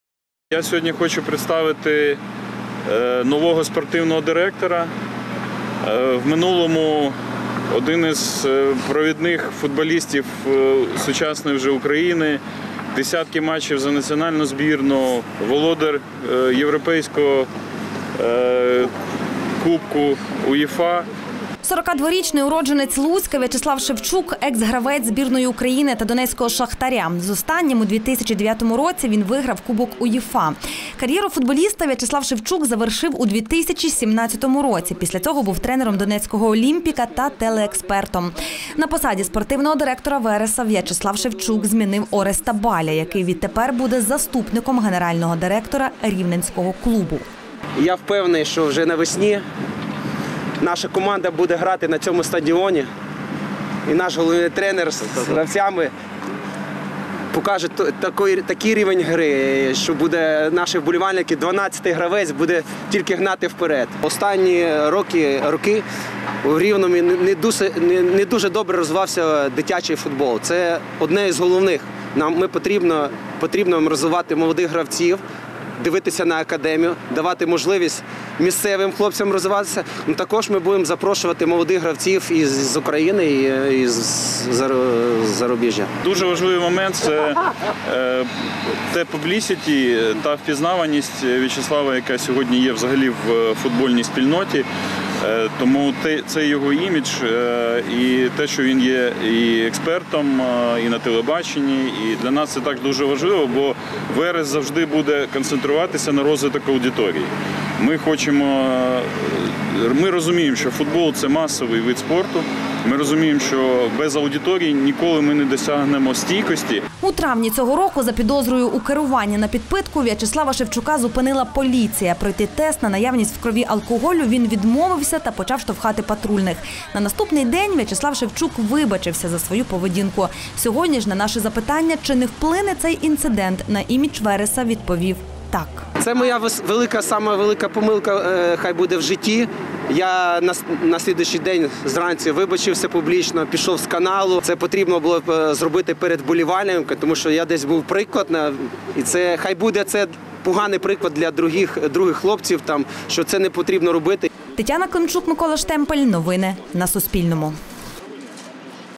У рівненського “Вереса” – новий спортивний директор. Що планує В'ячеслав Шевчук
У Рівному 23 листопада представили нового спортивного директора народного клубу "Верес" В'ячеслава Шевчука. На цій посаді він змінив Ореста Баля, який відтепер буде заступником гендиректора клубу. Під час представлення Шевчук заявив, що весняну частину сезону чемпіонату УПЛ "Верес" планує провести на рівненському стадіоні "Авангард".